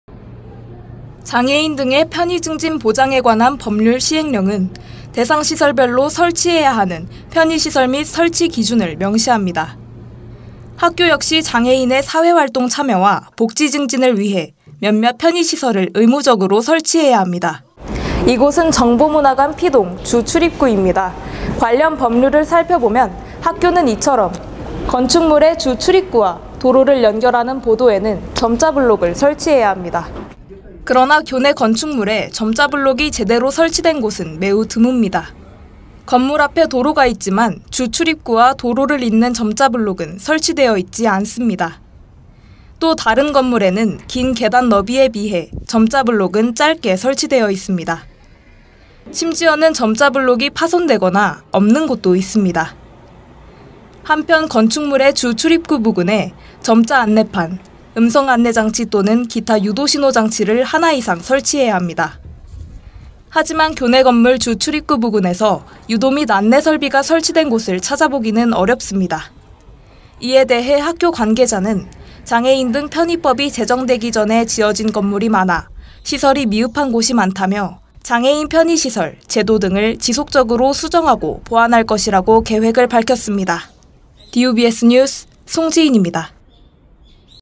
교내 장애인 편의시설 ‘미흡’
장애인 등의 편의증진 보장에 관한 법률 시행령은 대상시설별로 설치해야 하는 편의시설 및 설치기준을 명시합니다. 학교 역시 장애인의 사회활동 참여와 복지 증진을 위해 몇몇 편의시설을 의무적으로 설치해야 합니다. 이곳은 정보문화관 P동 주출입구입니다. 관련 법률을 살펴보면 학교는 이처럼 건축물의 주출입구와 도로를 연결하는 보도에는 점자블록을 설치해야 합니다. 그러나 교내 건축물에 점자블록이 제대로 설치된 곳은 매우 드뭅니다. 건물 앞에 도로가 있지만 주출입구와 도로를 잇는 점자블록은 설치되어 있지 않습니다. 또 다른 건물에는 긴 계단 너비에 비해 점자블록은 짧게 설치되어 있습니다. 심지어는 점자블록이 파손되거나 없는 곳도 있습니다. 한편 건축물의 주출입구 부근에 점자안내판, 음성안내장치 또는 기타 유도신호장치를 하나 이상 설치해야 합니다. 하지만 교내 건물 주출입구 부근에서 유도 및 안내설비가 설치된 곳을 찾아보기는 어렵습니다. 이에 대해 학교 관계자는 장애인등편의법이 제정되기 전에 지어진 건물이 많아 시설이 미흡한 곳이 많다며, 장애인 편의시설, 제도 등을 지속적으로 수정하고 보완할 것이라고 계획을 밝혔습니다.

DUBS뉴스 송지인입니다.

REP 송지인
ENG 정민경
AD 김희정
제작 DUBS